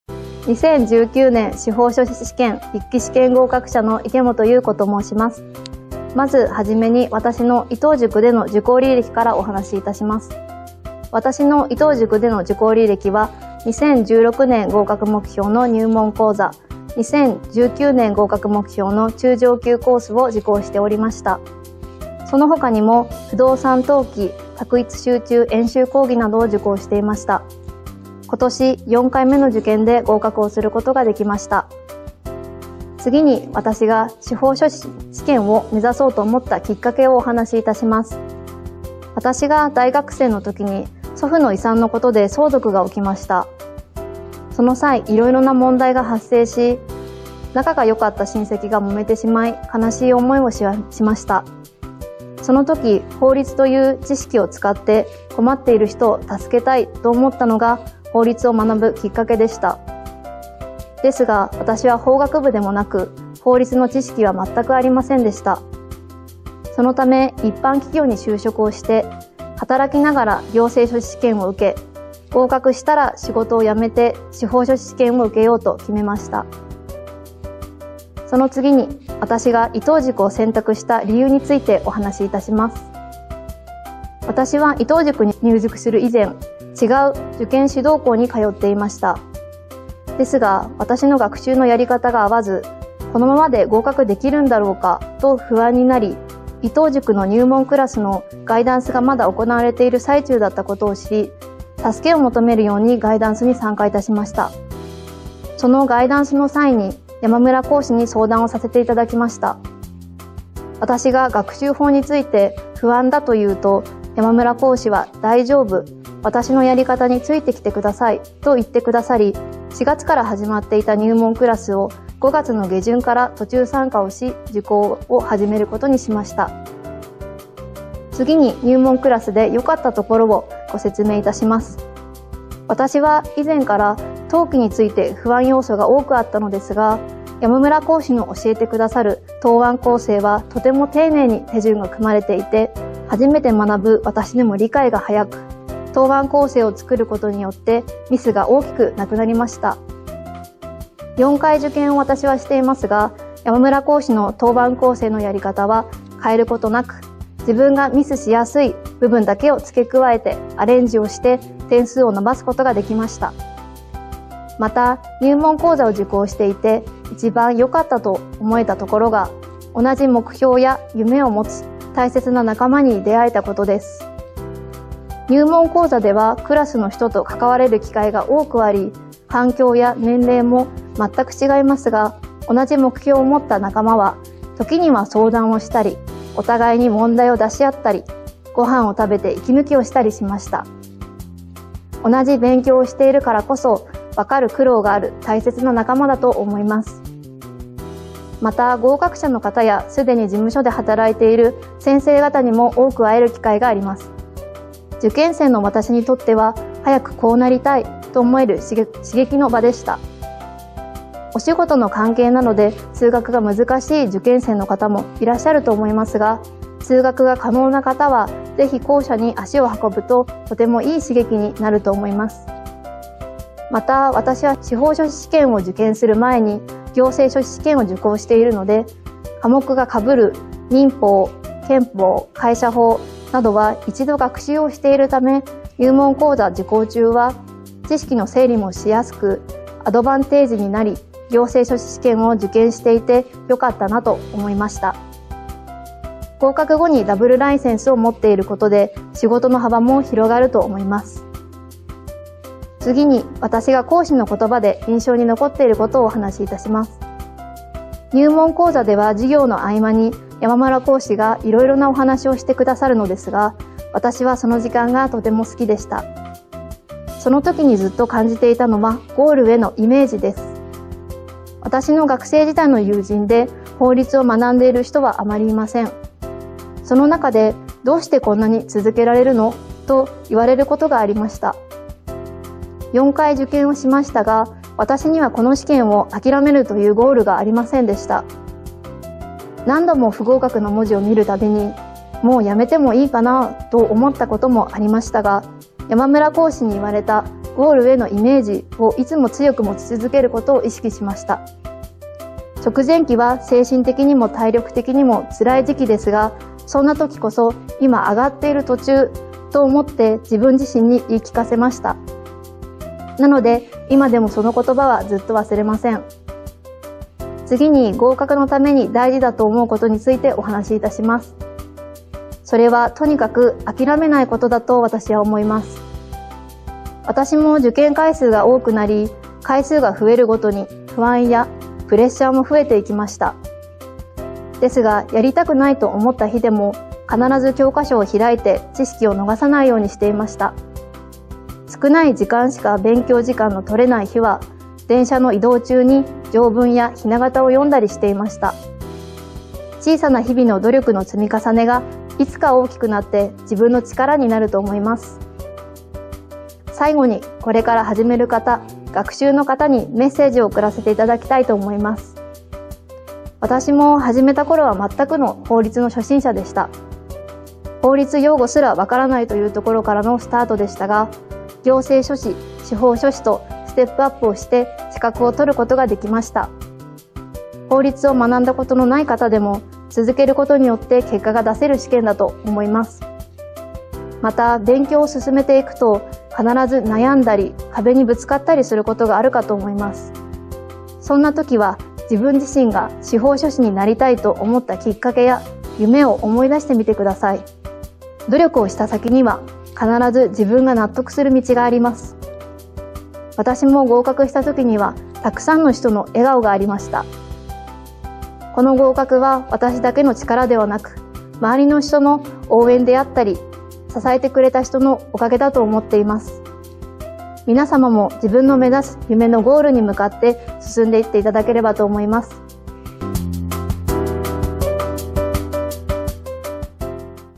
2019年司法書士試験合格者メッセージVol.2～他校からシフトし、安心の入門講座で合格～
2019年度司法書士試験に見事合格を果たしたみなさんに、その体験談をお話いただくシリーズです。
今回は勉強方法に悩んでいた他校の講座から入門講座にシフトし合格された池本優子さんに学習法をお話いただきます。
司法書士試験に合格する方と、長くかかってしまう方とは何が違うのか？
合格のために本当に効果的な合格方法は？
私のやっている勉強方法は間違い？独学との違いは何か？
合格者から学べることはたくさんあります。合格方法は多種多様ですが、このメッセージから自分にあった合格法がみつかるかもしれません。
合格者の語る合格の秘訣を参考に、皆さんも来年度の合格を目指してください！
▼有益な情報が満載！司法書士試験合格者の声